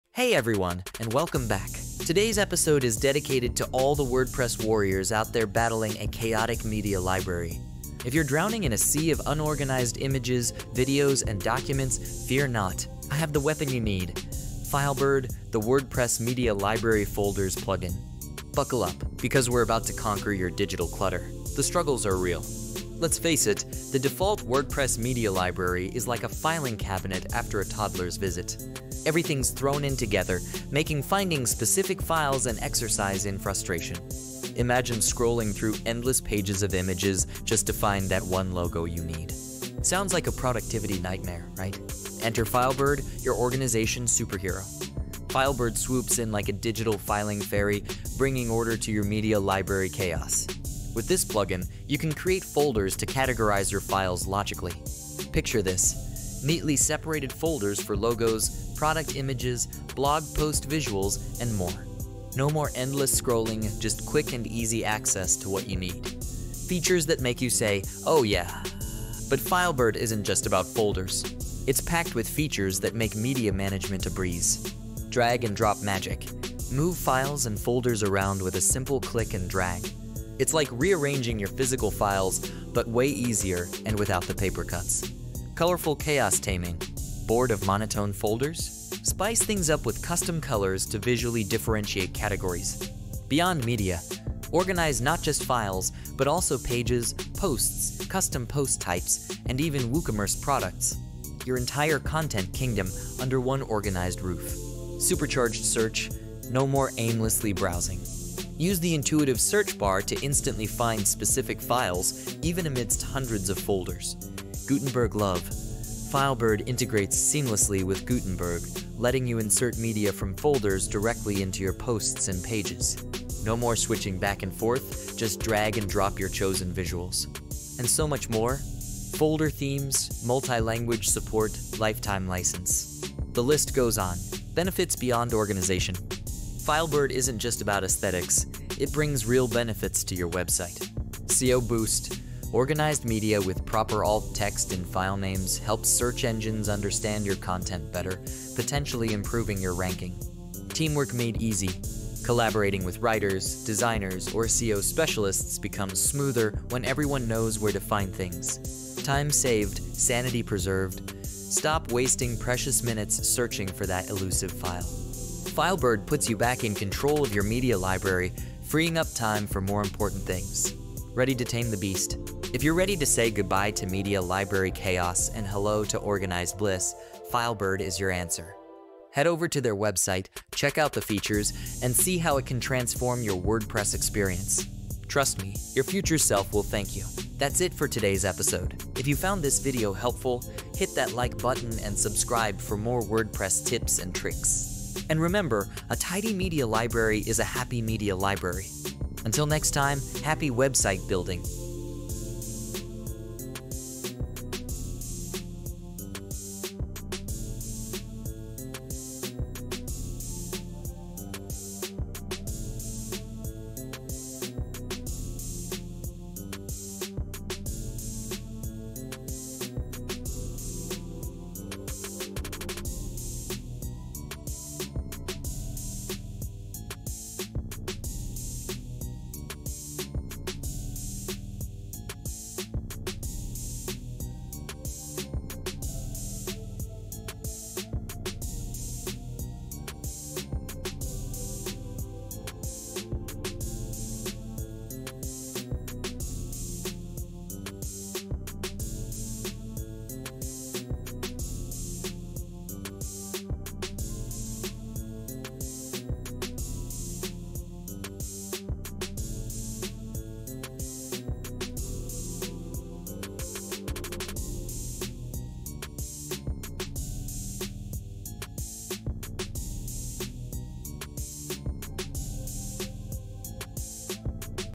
how to organize wordpress files in media library folders - EvaluTech
If you're drowning in a sea of unorganized images, videos, and documents, fear not! I have the weapon you need: FileBird, the WordPress Media Library Folders plugin. Buckle up, because we're about to conquer your digital clutter!

The Struggles are Real

Let's face it, the default WordPress media library is like a filing cabinet after a toddler's visit. Everything's thrown in together, making finding specific files an exercise in frustration. Imagine scrolling through endless pages of images just to find that one logo you need. Sounds like a productivity nightmare, right?